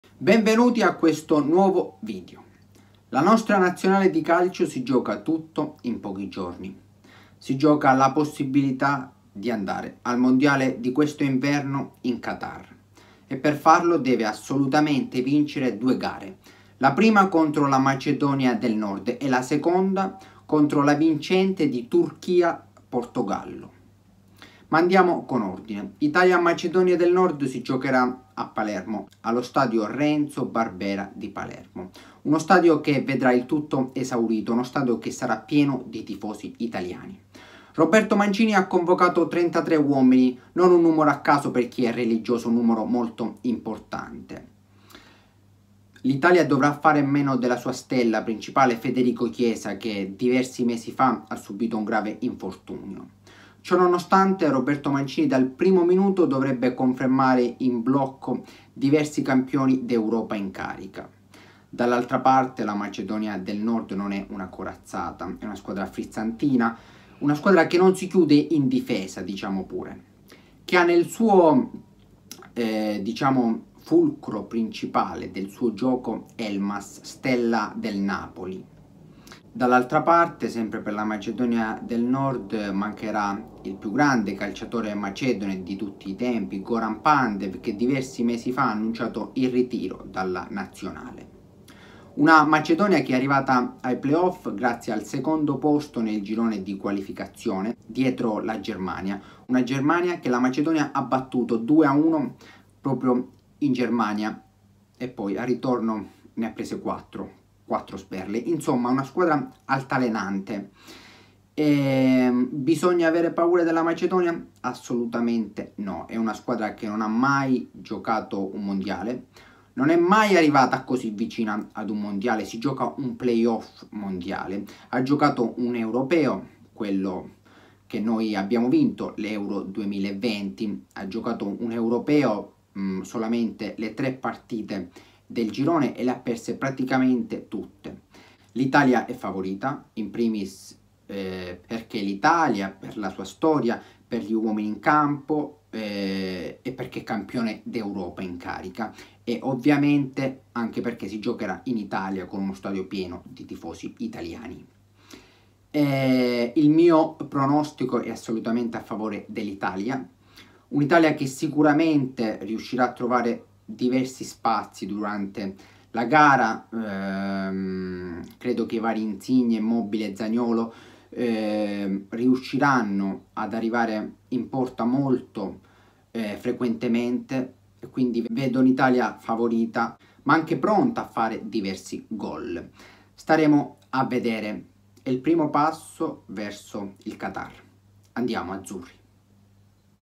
Analisi e Pronostico di ITALIA vs MACEDONIA DEL NORD!!!
Per la nostra Nazionale è arrivato il momento. In pochi giorni ci giochiamo il mondiale, due gare da dentro o fuori che bisogna vincere.
Si inizia con Italia Macedonia del Nord.